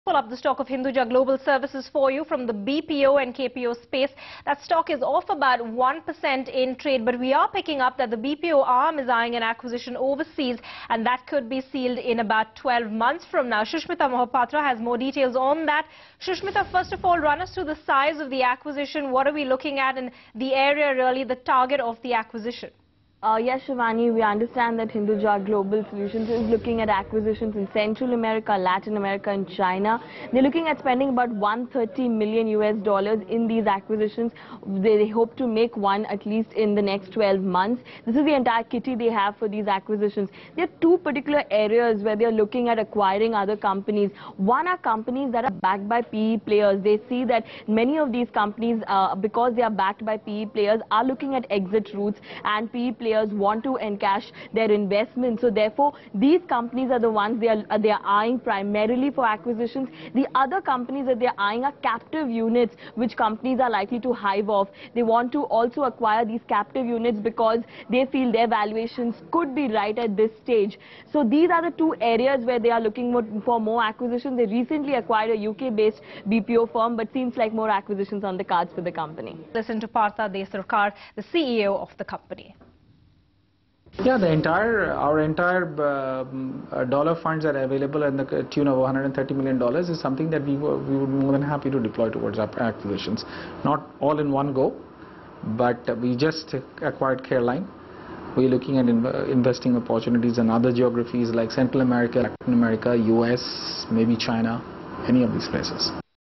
Hinduja Group to buy properties abroad
According to sources Hinduja Group is eyening overseas market to buy properties...